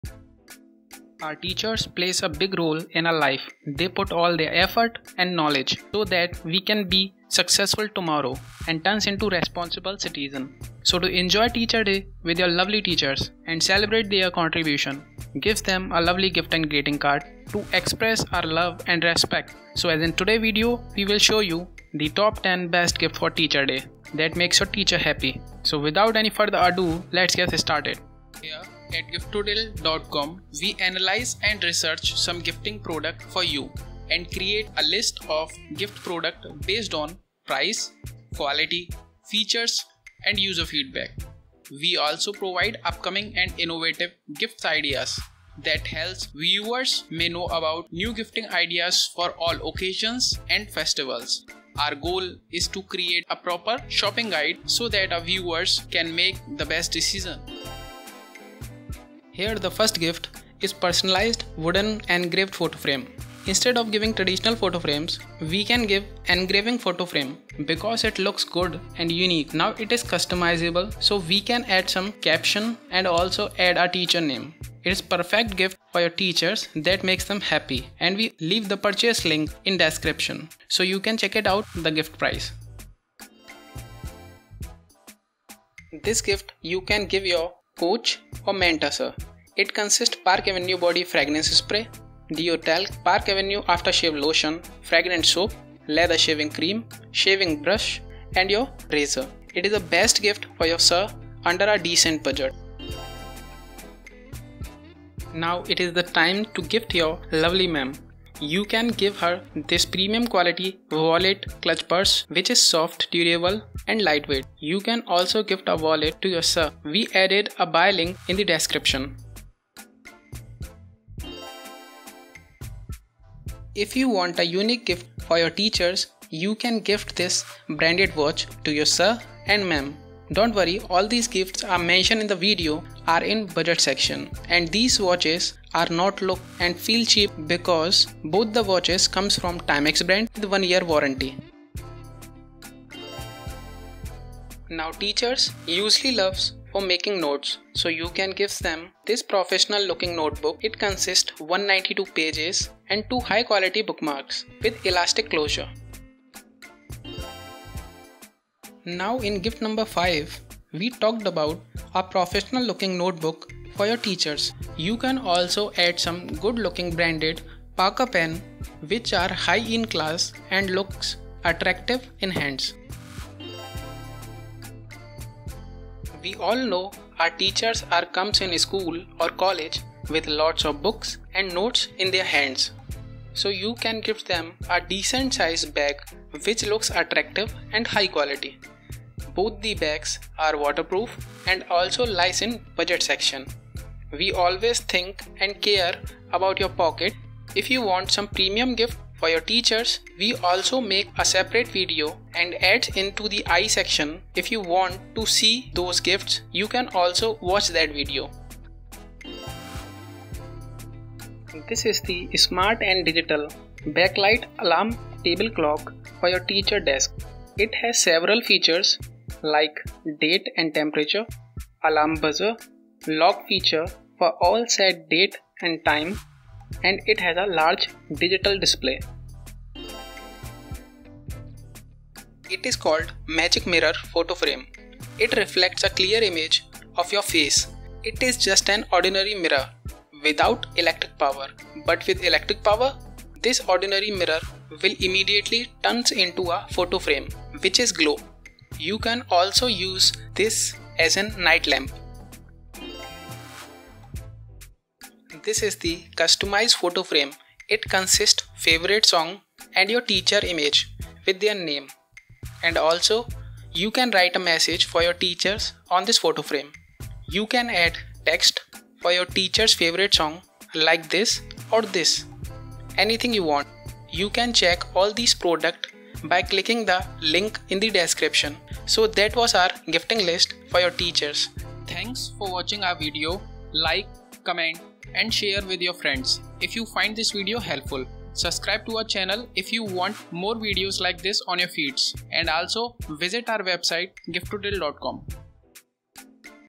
Teachers Day Gifts🧑‍🏫👨‍🏫 | 🎁Giftodil.com
In this video, you are watching the  Top 10 Best Teachers Day Gift in Budget | 🎁Giftodil.com

00:00 - Introduction

00:58 - Personalized photo frame -- Incredible Gifts Personalized Photo Plaque-Gift for the teacher.

01:24 - Park Avenue Grooming Set -- Park Avenue Luxury Grooming Collection (Combo of 7 + Travel Pouch).

01:44 - Wallets (Female) -- PALAY® Women's PU Leather Mini Short Card Holder Small Wallet Clutch Purse, Pink.
                               (Male) -- Urban Forest Oliver RFID Blocking Blue Leather Wallet for Men - Packed in Premium Wooden Box for Gifting.

02:03 - Watch (Female) -- Gully by Timex Swirl Analog White Dial Women's Watch.
                              (Male) -- Timex Classics Analog White Dial Men's Watch

02:25 - NoteBook -- Papercoal Notebook with Buckram Hard Cover - Cranberry- Unruled.

02:38 - Parker Pen -- Parker Classic Gold GT Ball Pen.

02:55 - Bag (Male) -- Veneer Men's Synthetic Leather 15.6 Inch Waterproof Large Messenger Bag
                         (Female) -- Fristo Women's Handbag

03:32 - Table Clock -- Case Plus Digital Smart Backlight Battery Operated Alarm Table Clock with Automatic Sensor, Date & Temperature (Black) (Black Alarm Clock)

03:52 - Light Photo Frame -- JIADA Magic Mirror Photo Frame| Magic Mirror LED Photo Frame

04:15 - Customized Photo Frame -- Shaivya Acrylic Customized Photo Printed Musical Plaque, Personalized Spotify Plaque with Scannable Code and your Photo

***The following video abides by the Youtube Community Guidelines.